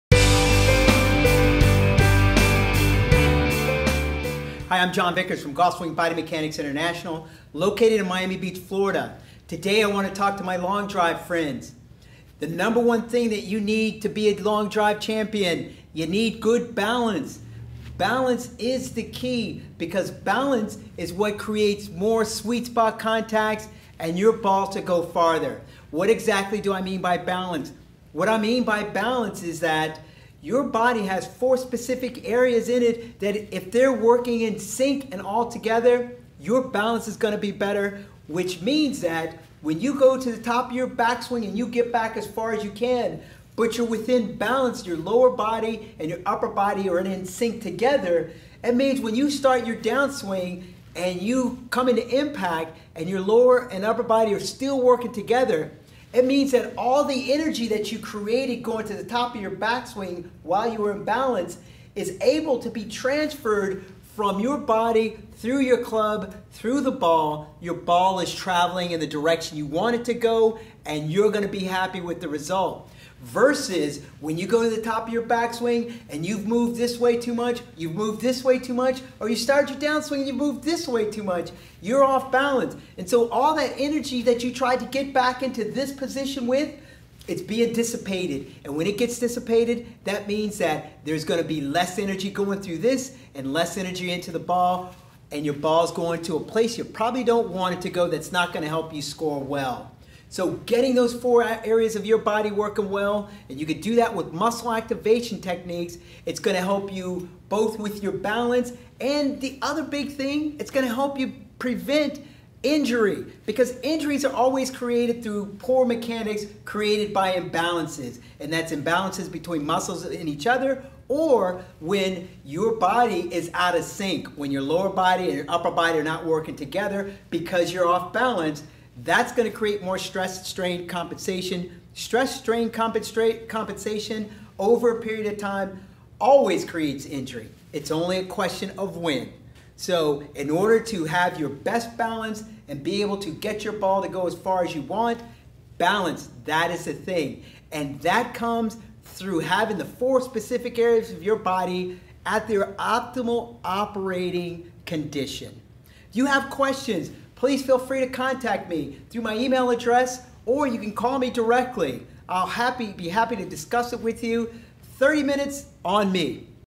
Long Drivers MUST see Video, John Vickers Muscle Activation Techniques Specialist, Miami Beach
This is a Long Drivers must see video talking about the Nr. 1 thing ( Your Balance)